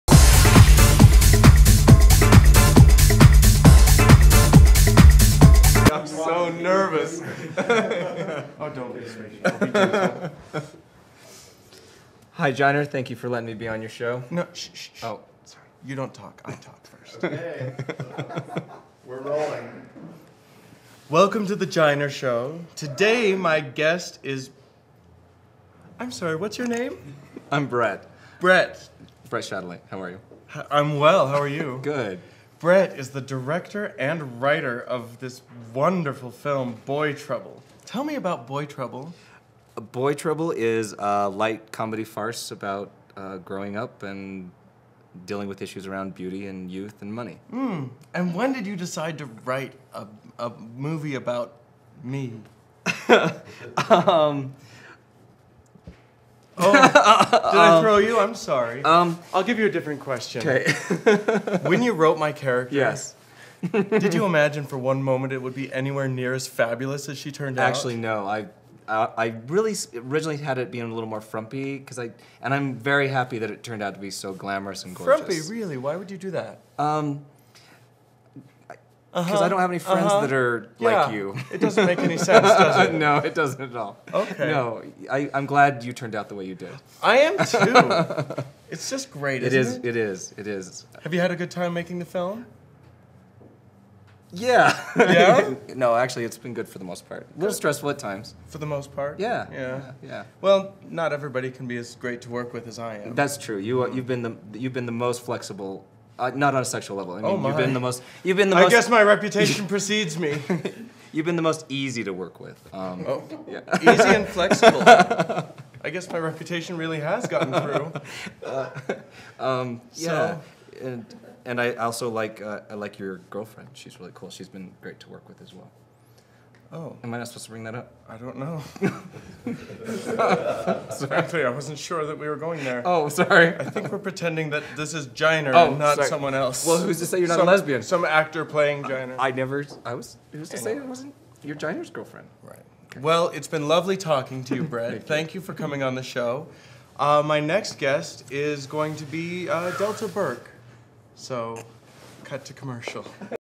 The Giner Show
This is Giner Show hosted by the famous Giner.  In this episode, Giner interviews me.  Take a look.